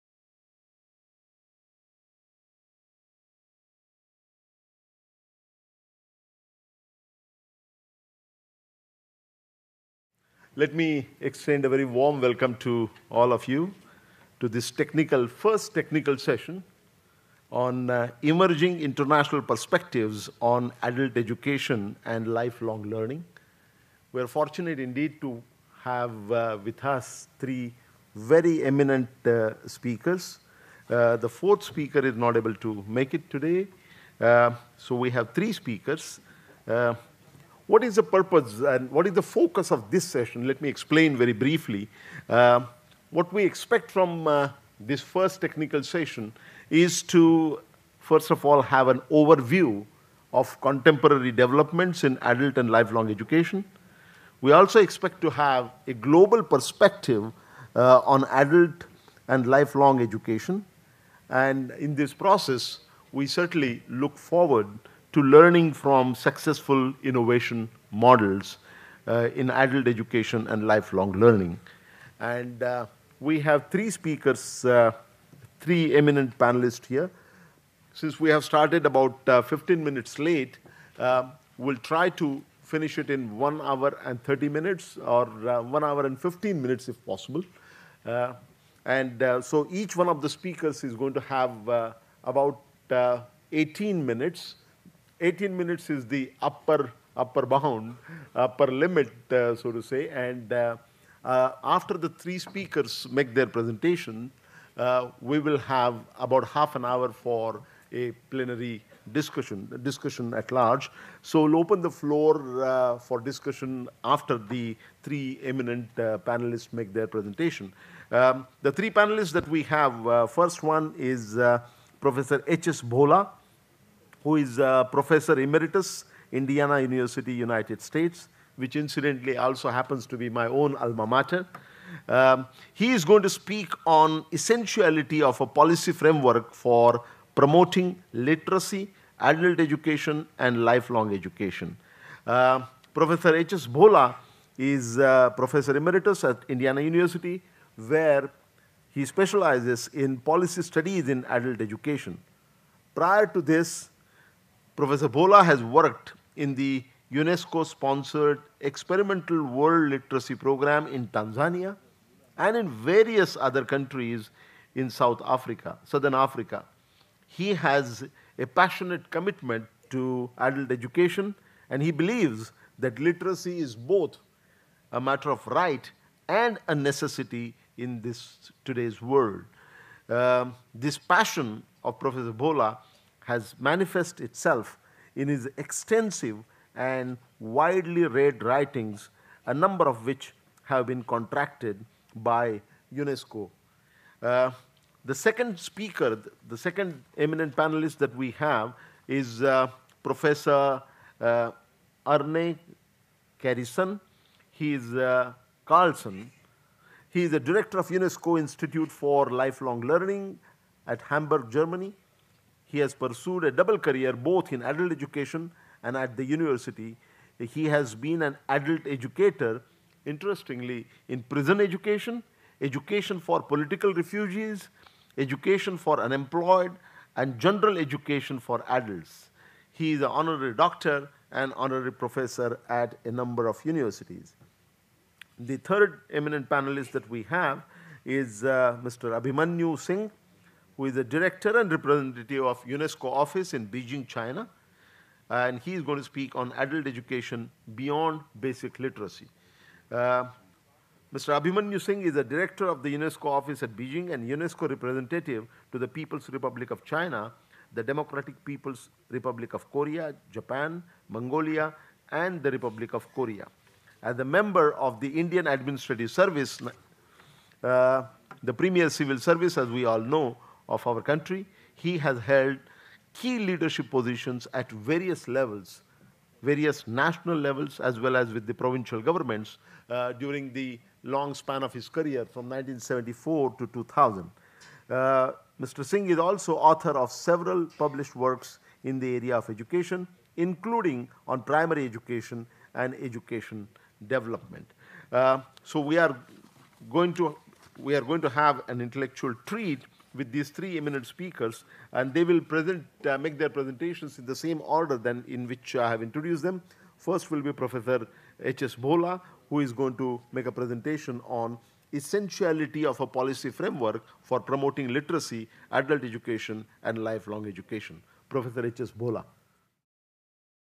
Dr Narendra Jadhav (2011), Women's Literacy, Chair, Technical Session in Intl Conference, New Delhi
Dr. Narendra Jadhav, Chairing a Technical Session in International Conference on 'Women's Literacy for Inclusive and Sustainable Development' , UNESCO & Saakshar Bharat,New Delhi Septmber 9, 2011.